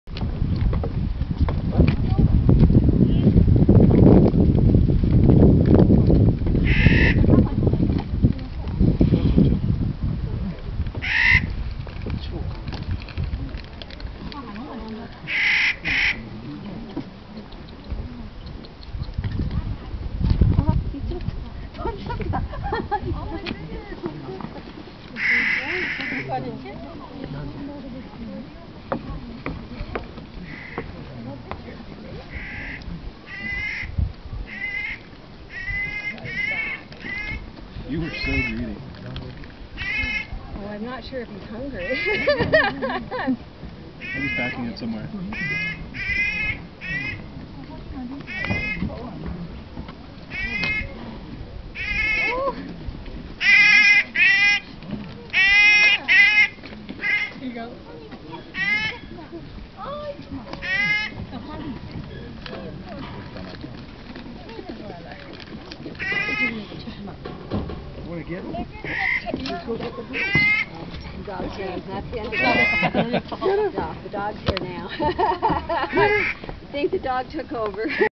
Feeding chipmunks at Sulphur Mountain
This guy was feeding nuts of some kind to the chipmunks on Sulphur Mountain top, in Banff, AB. Taken in late June 2009